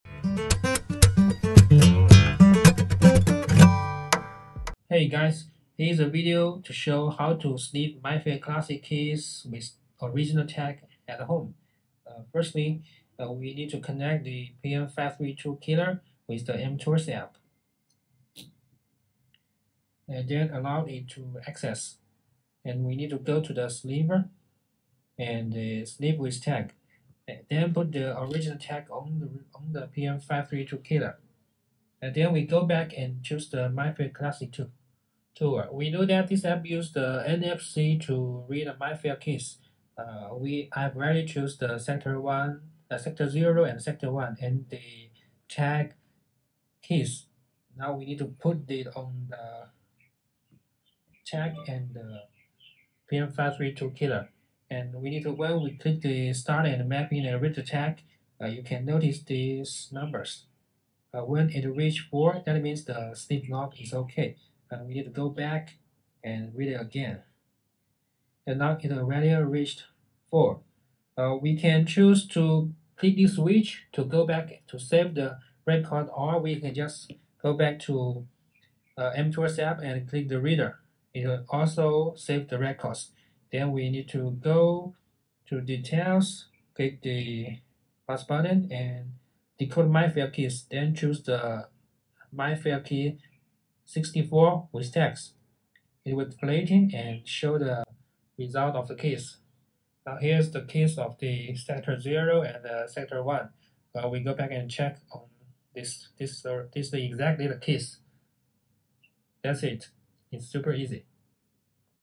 Mfkey64 Sniff with Tag on PN532Killer
PN532Killer has 2 sniff modes which include Mfkey32v2 and Mfkey64.
This video shows how to sniff with a tag, that's Mfkey64.

00:24 Enter Sniff mode
00:35 Use MCT to read the tag
01:19 Mfkey64 record fetched
01:34 Decode Mfkey64 in MTools